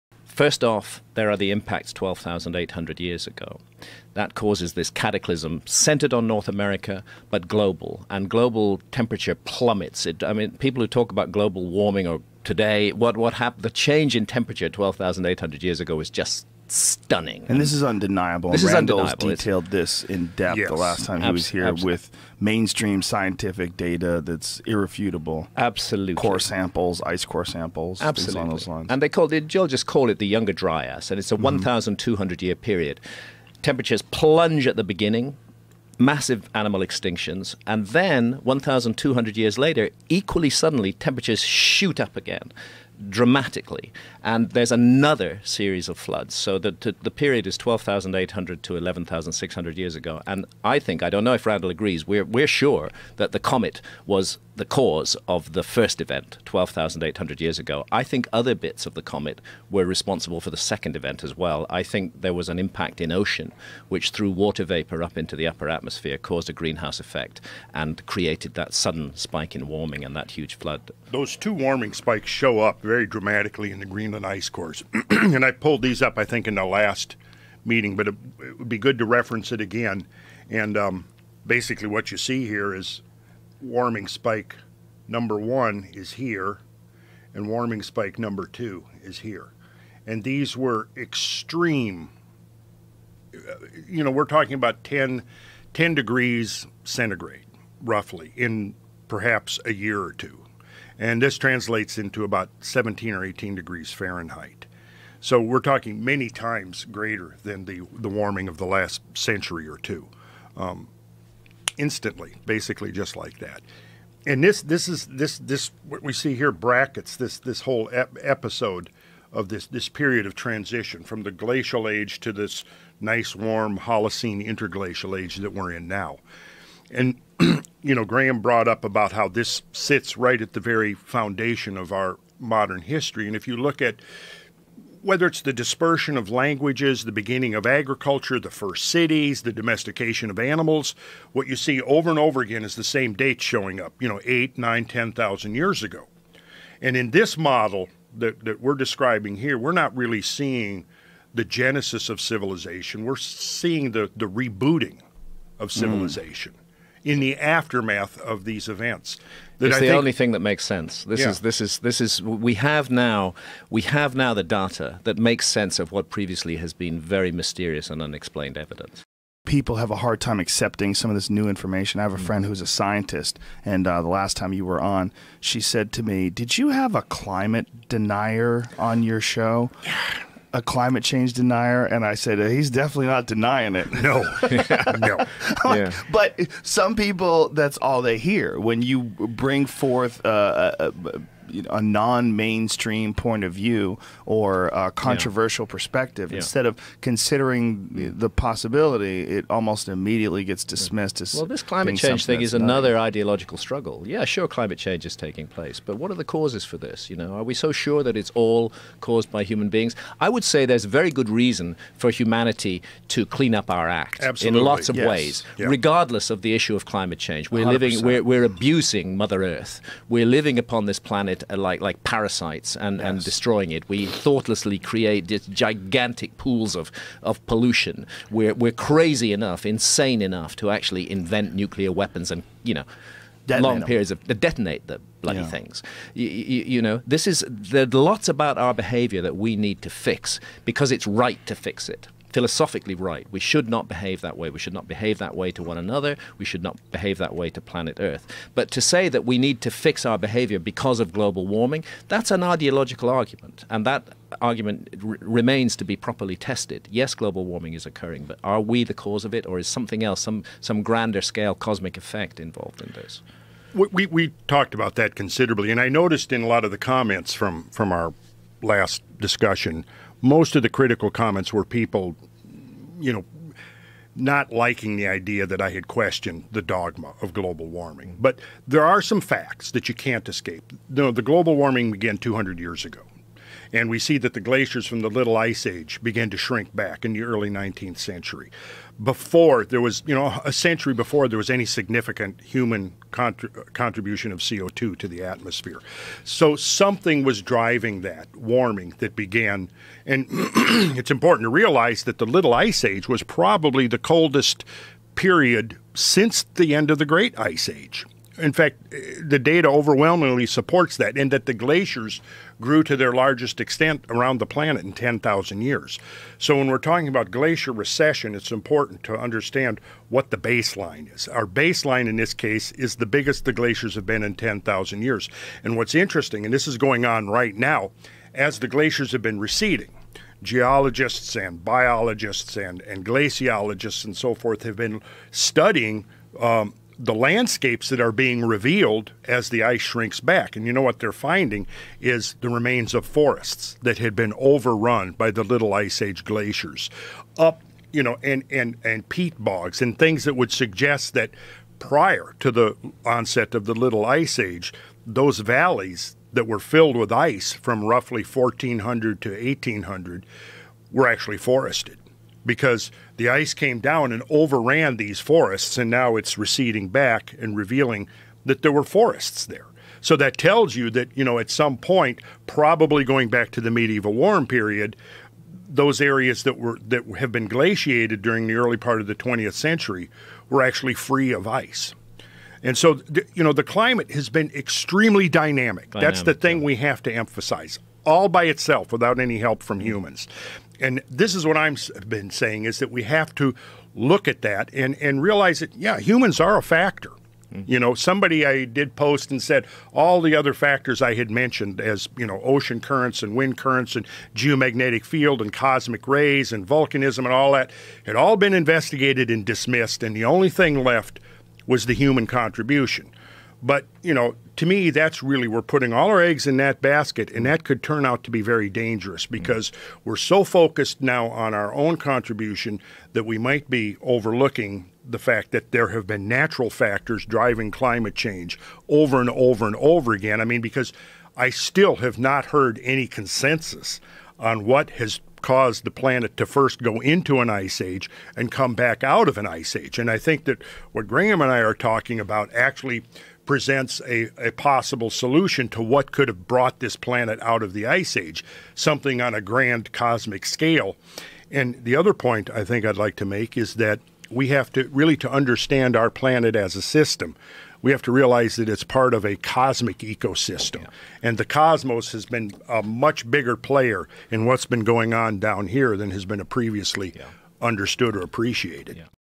Climate Change The REAL Inconvenient Truth!! NOT man made PART 2/2
Via Information Warfare

Stop blaming humanity! Climate change is NOT man-made! This is the REAL Inconvenient Truth.

Graham Hancock talks about the massive comet impact that caused this massive climate change that happened 12,800 years ago.

Randall Carlson walks us through the evidence of climate change found in oxygen isotopes in Greenland. Randall also replies the climate critics.